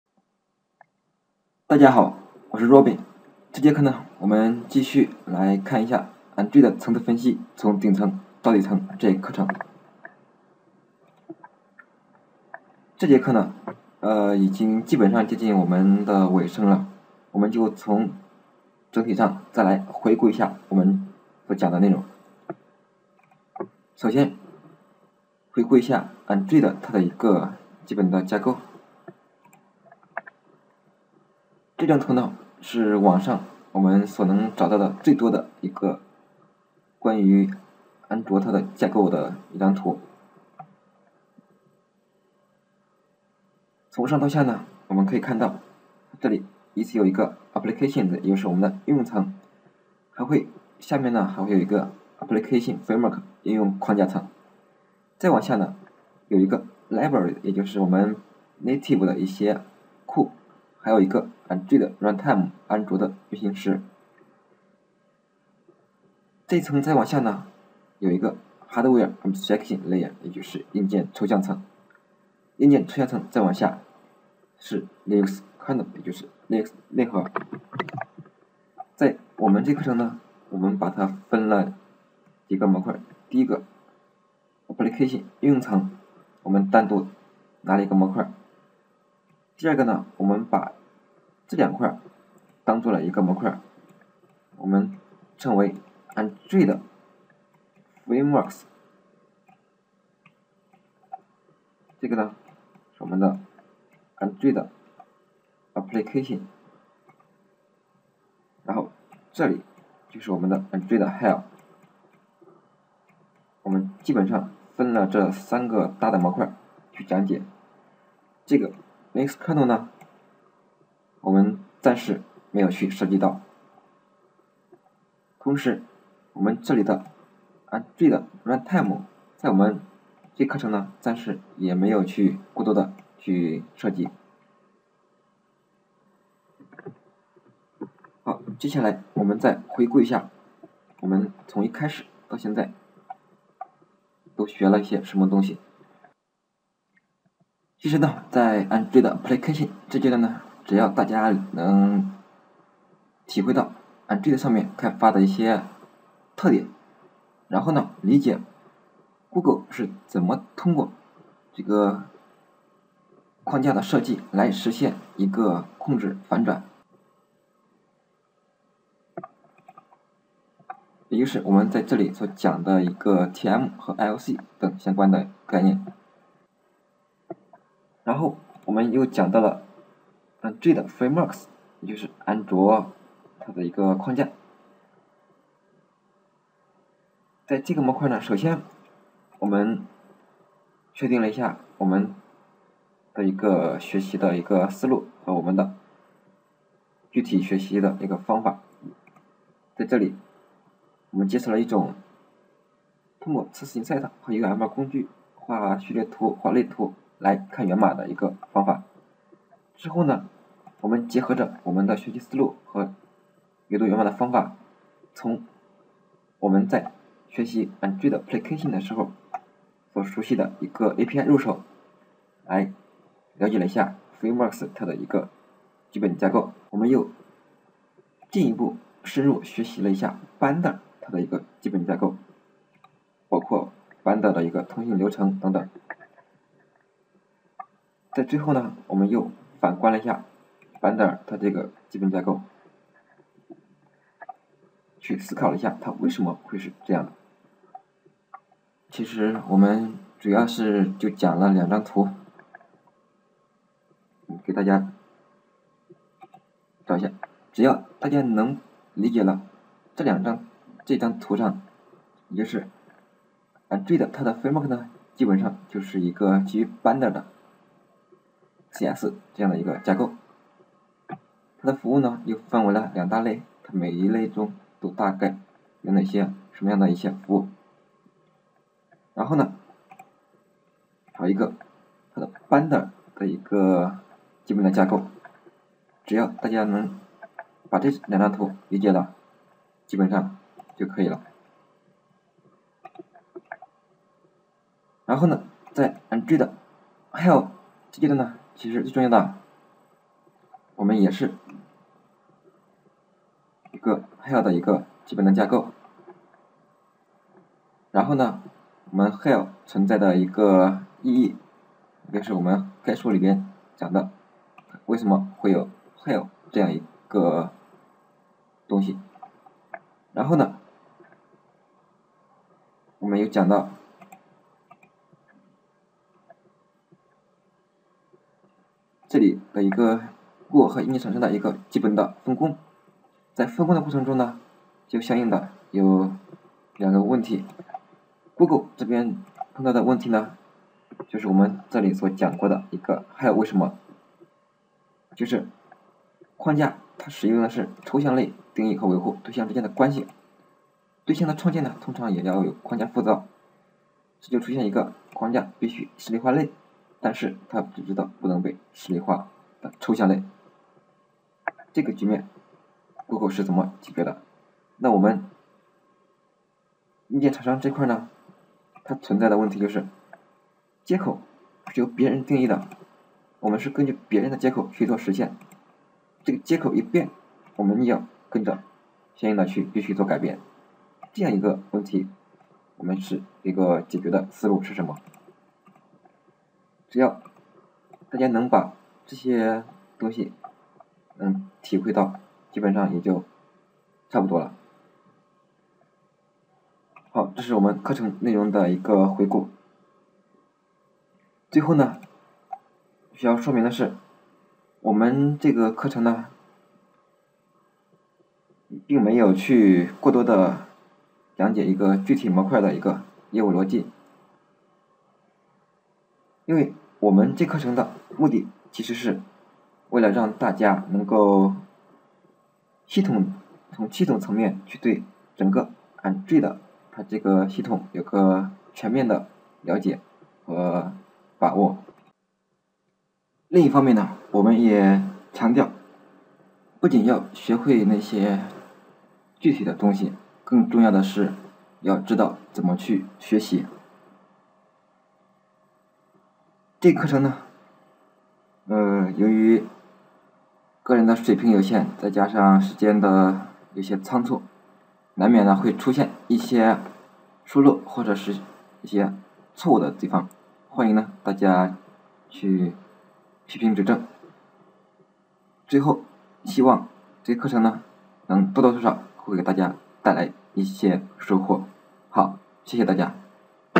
Android层次分析 –从顶层到底层-33.整体再回顾
本课程通过结合实例代码来分析Android是怎么由应用一步步到底层的，以把Android OS纵向地串接起来。同时会把学习的思路融入课程，让你不仅仅知道答案是什么，更进一步知道怎么去获取答案。

在课程结束的时候你将知道从应用到底层分别是什么样的，以及是怎么一层层下来的。可以让你对整个Android系统有个直观的认识，为日后根据个人兴趣选择进入Android各层开发打下基础。